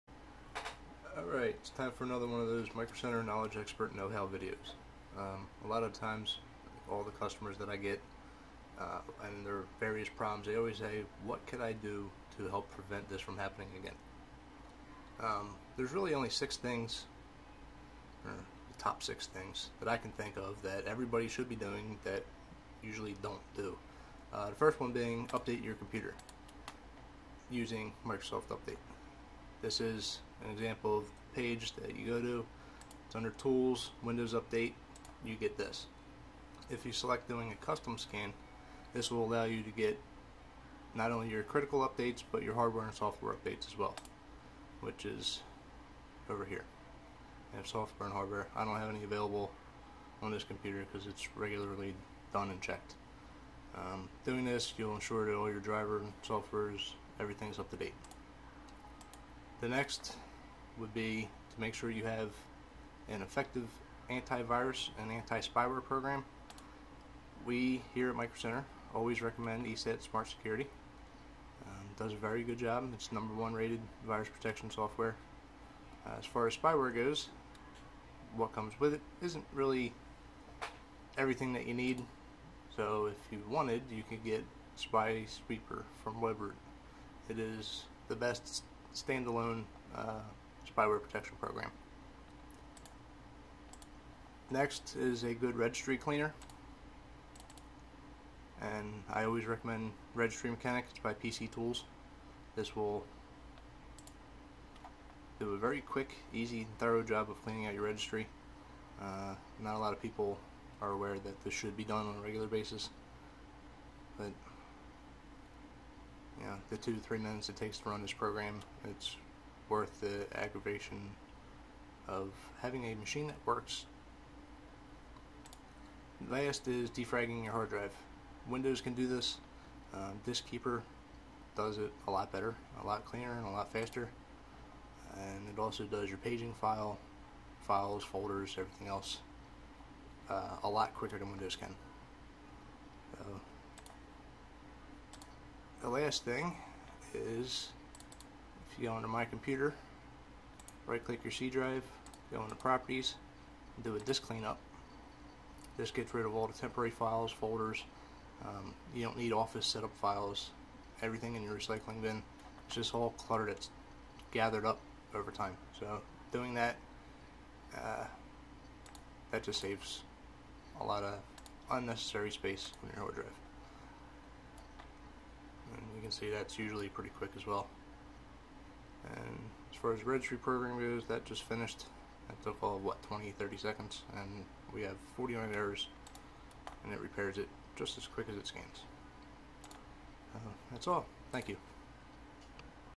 Tech Support: Troubleshooting Computer Problems
Micro Center Tech Support shows you how to prevent computer problems by following a few recommendations. This video covers OS updates, anti-virus, and registry cleanup.